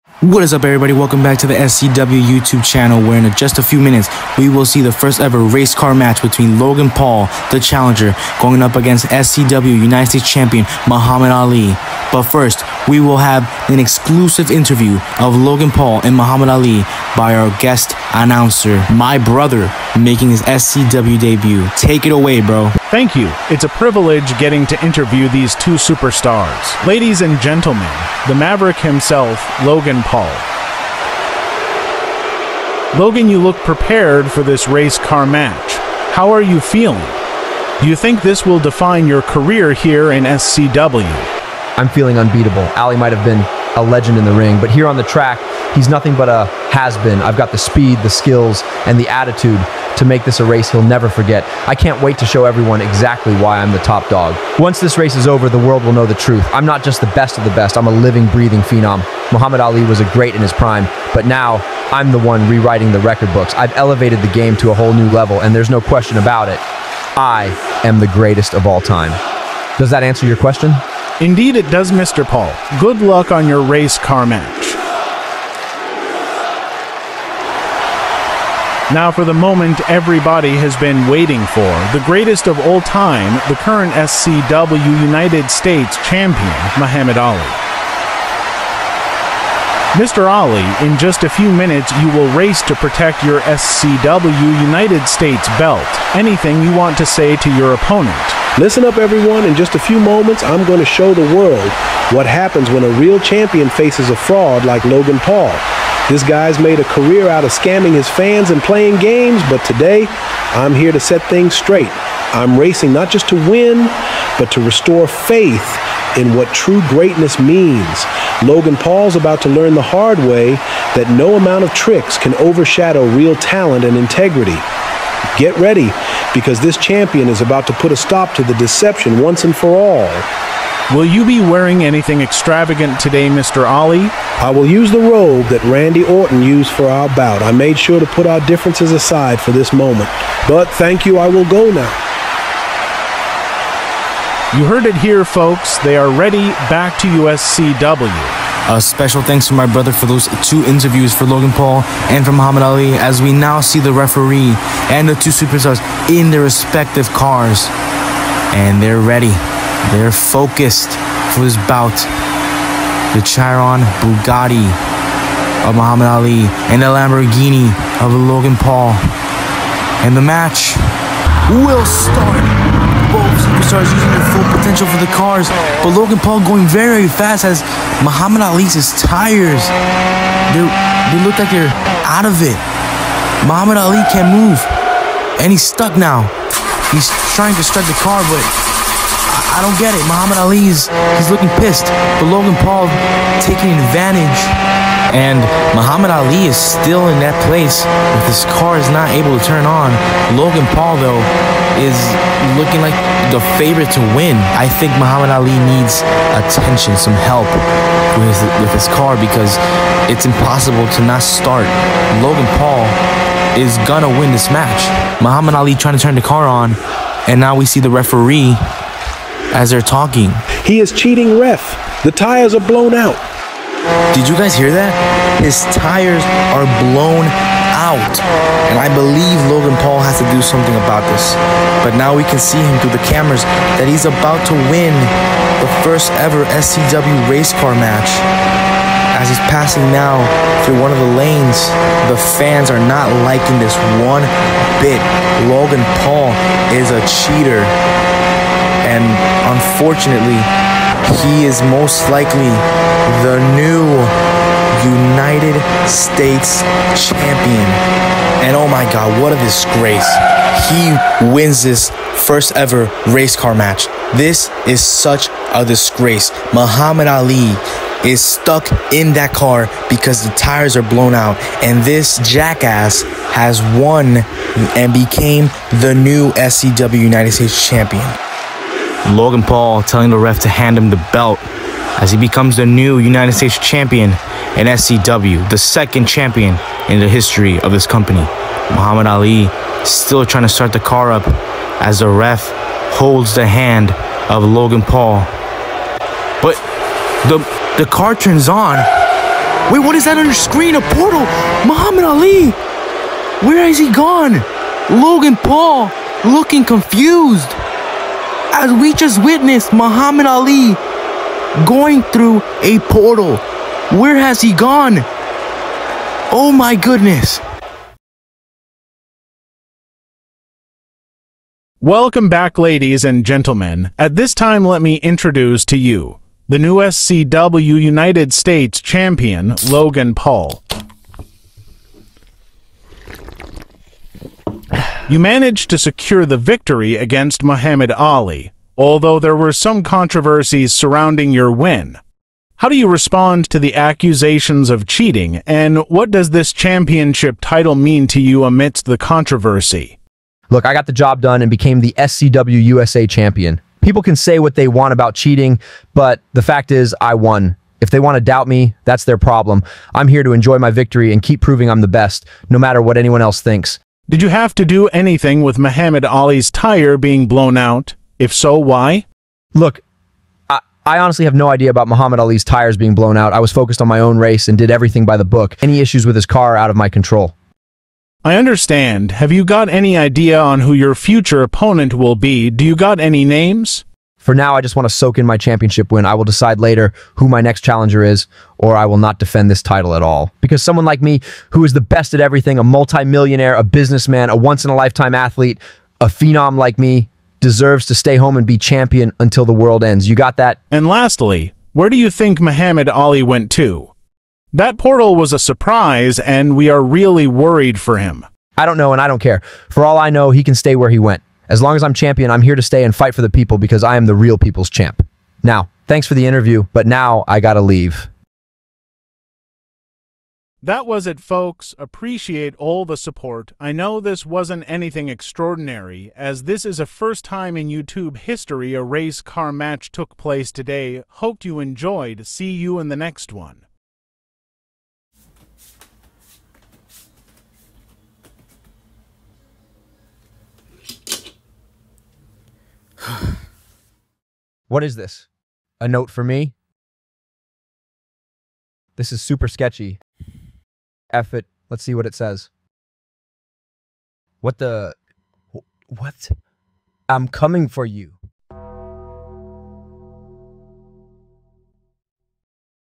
MUHAMMAD ALI VS LOGAN PAUL RACE CAR MATCH FOR THE SCW UNITED STATES CHAMPIONSHIP! | SCW WRESTLING
MUHAMMAD ALI FACES LOGAN PAUL FOR THE SCW UNITED STATES CHAMPIONSHIP IN THE FIRST EVER RACE CAR MATCH!

🛑DON'T GET HYPED! only 1 lap due to the hot weather and 64 gb on my phone🛑